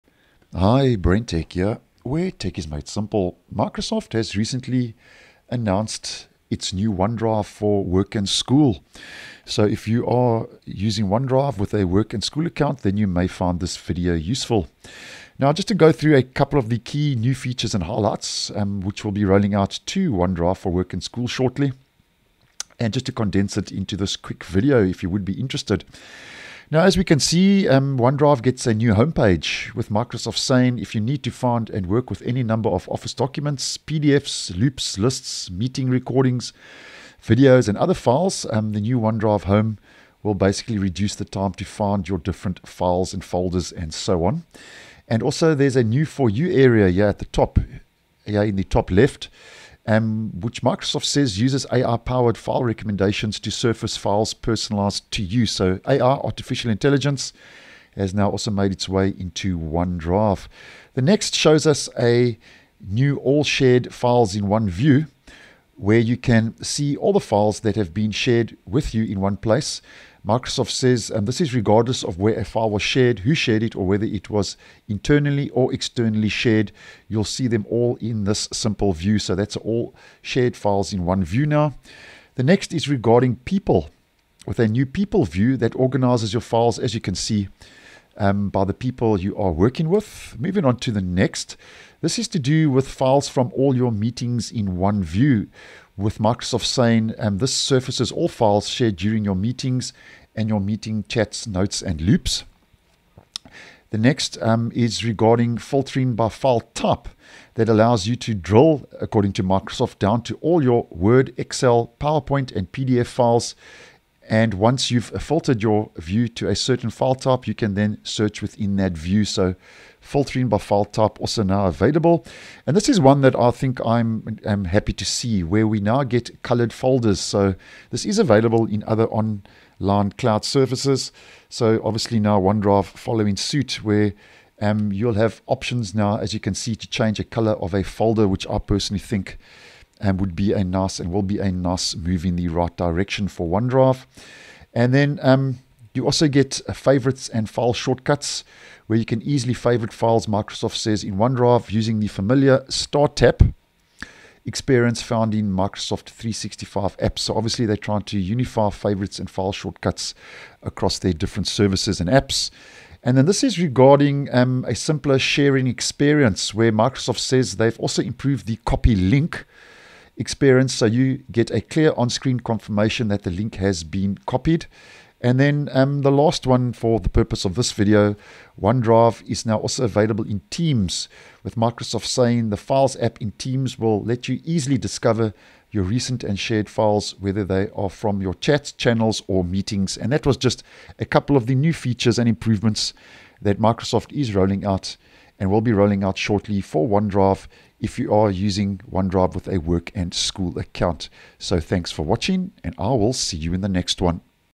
Microsoft OneDrive for work and school is getting a redesign and new features shortly
Microsoft has announced the new OneDrive for work and school that includes a new OneDrive Home, all shared files in one view, a new People view, Files from all your meetings are now in one view, filtering by file type, Colored folders and more.